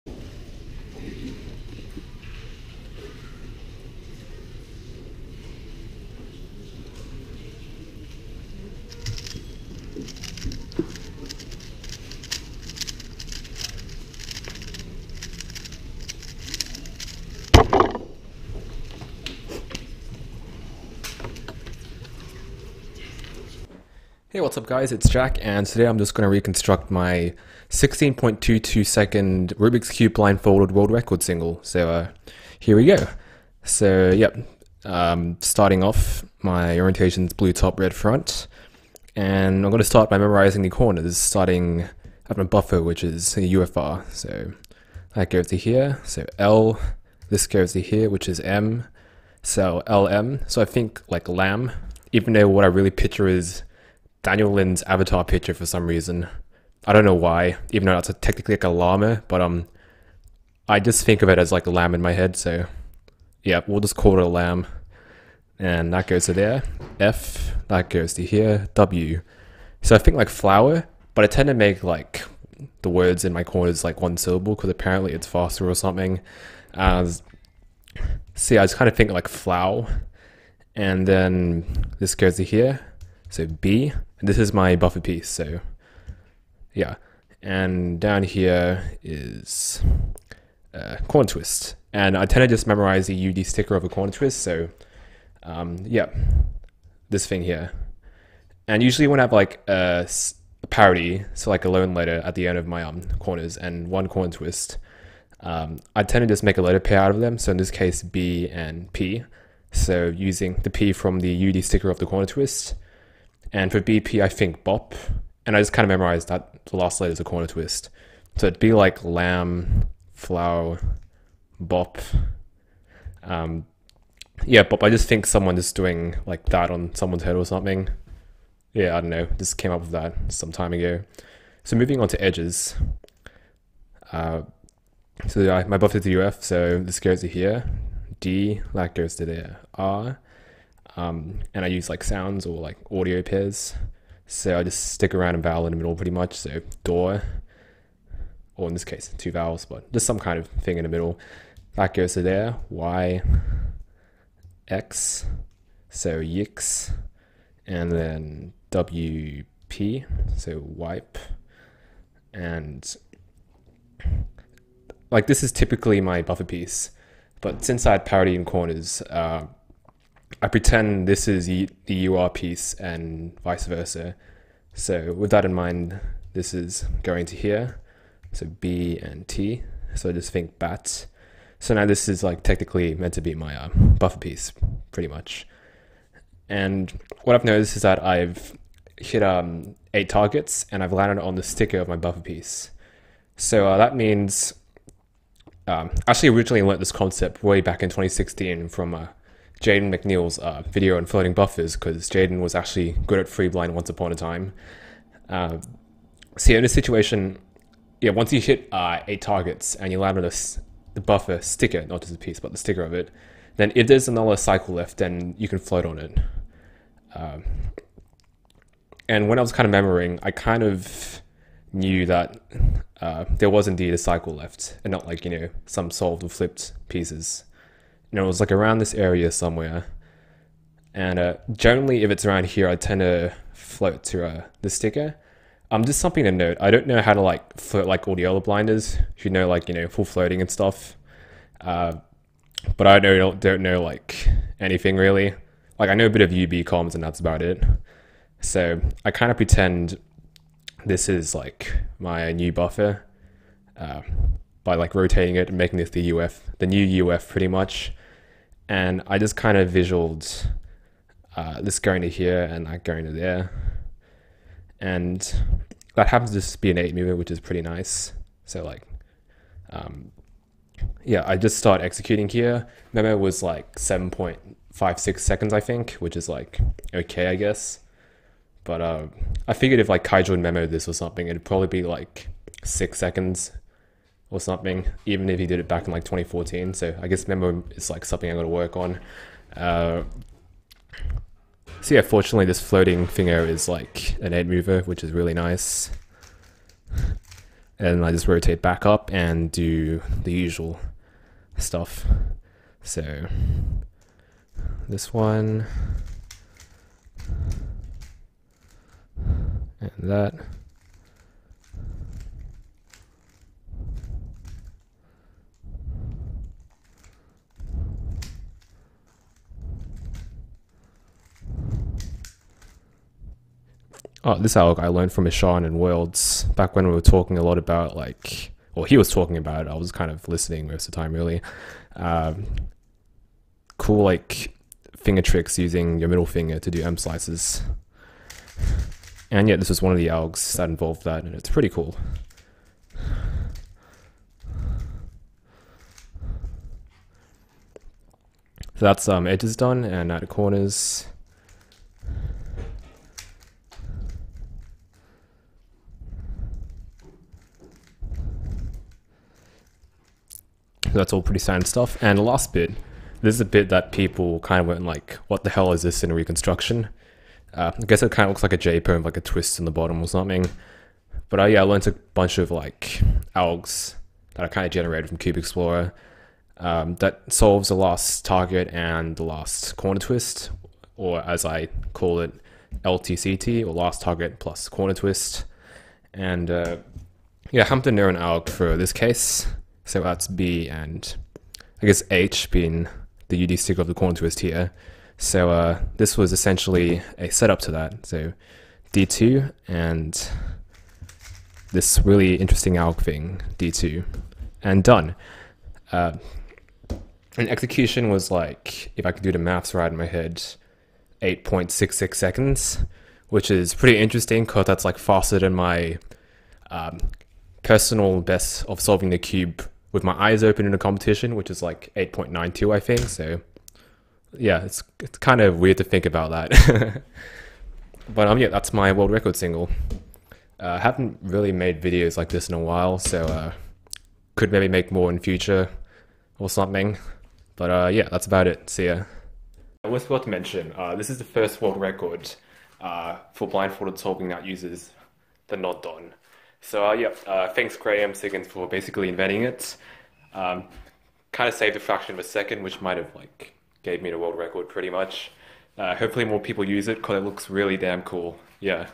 3x3 Blindfolded WR Reconstruction -16.22 seconds (Former)
Want to take your blindsolving to the next level?

kinda spoke a bit too close to the microphone w00ps, pls reduce volume a bit or else get blasted with some weird heavy breathing 👌

Scramble: R F2 L D2 L' B2 L2 U2 R' B' R' F2 L' B' L D' B2 L' D2 U Rw2 Uw'

=Memo=
Corners: LaMb FloW(er) Bo[P]
Edges: DooR YiX WiPe BaT [visual x to DP alg]

=Execution=
x2 z’
[x: [U, L E’ L’]]
[Uw’: [R’ E R, U’]]
[U L2 U’, M]
[M’: [U R U’, M2]]
[M: [U M’ U’, R’]]

[R U: [U, R’ D R]]
[z: [D2, R U R’]]
D2 R D' R' U2 R' U' R2 D r' U2 M’ D2

Cube: Gan Air SM
Camera: GoPro Hero Silver 4

📦 Some stuff that I use 📦
🎵 Otamatone (very important)
Camera (newer version)
Laptop (newer version)